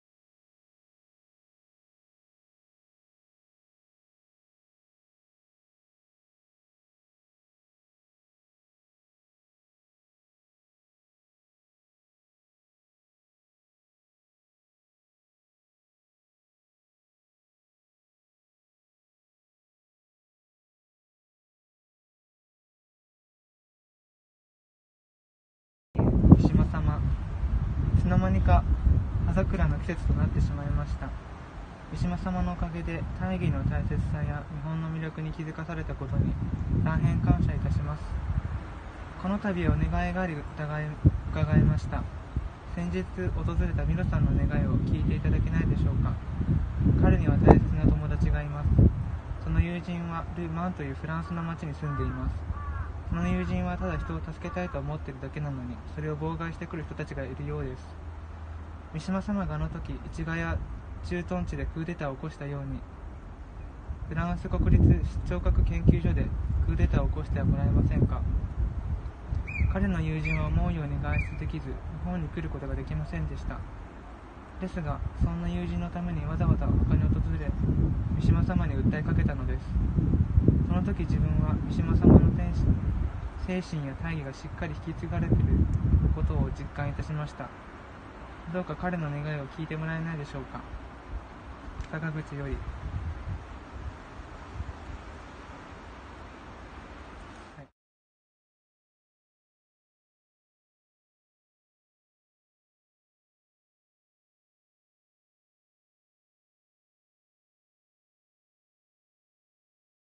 L'INA face à Mishima
Mon ami voyageur Benoît a eu vent, depuis l'autre bout du monde, de mon procès avec l'INA

(dont les avocats me réclament la somme de 158 023 € pour avoir créé un blog contre eux, en y publiant leurs propres archives).

Il a tenu à adresser une lettre à Mishima, sur sa tombe, au cimetière de Tama, à Tokyo.

L'écrivain, depuis le départ, est lié d'une façon merveilleuse et mystérieuse à cette mésaventure.

Benoît ne parlant pas japonais, il demande à un garçon rencontré sur place, Ayumu, handicapé des jambes, de lui traduire le texte, et de le dicter devant la tombe.

La traduction, de l'anglais au japonais, puis du japonais au français, peut comporter quelques approximations.

Je remercie Benoît et Ayumu.
Prochaine audience le 11 avril 2018.